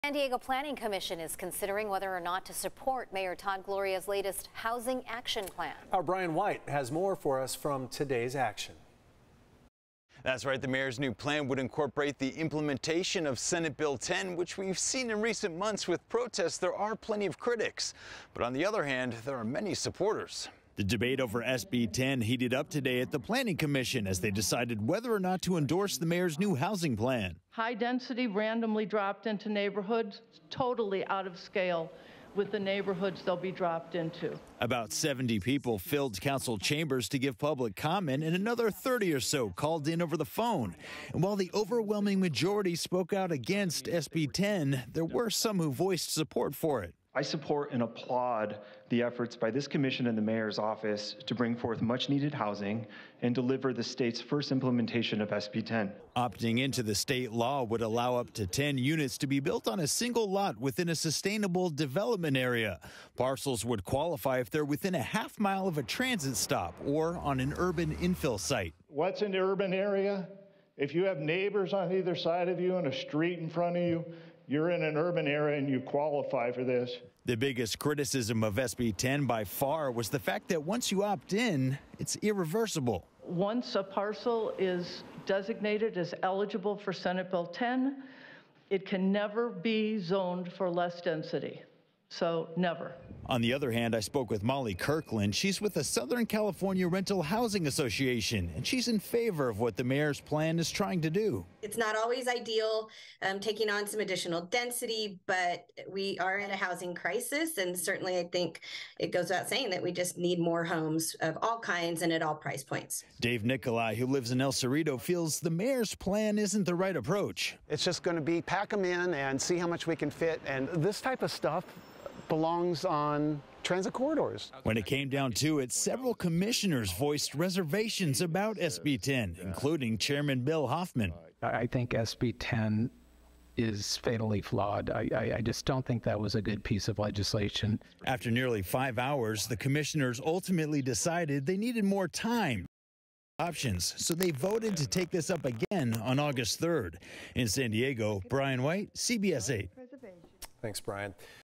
Debate over SB 10 heats up with Mayor Gloria’s Housing Action Plan 2.0
During a five-hour meeting, the Planning Commission considered Mayor Todd Gloria’s latest Housing Action Plan Thursday. The plan would incorporate the implementation of Senate Bill 10, the prominent source for debate in council chambers during public comment.